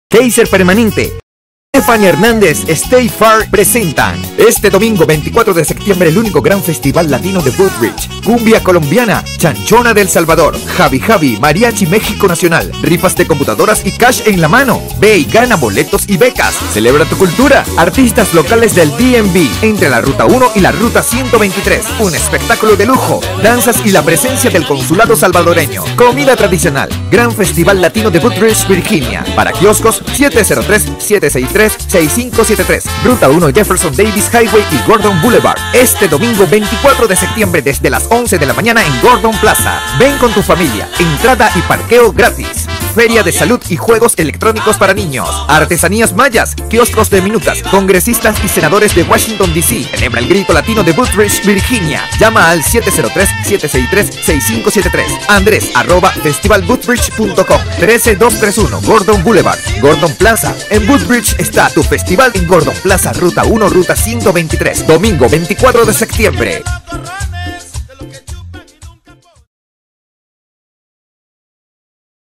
Gran Festival Latino de Woodbridge 24 Septiembre
Festival de Woodbridge, Sunday 24 September
11 AM - 6 PM
GORDON PLAZA, Entre Ruta 1 y Ruta 123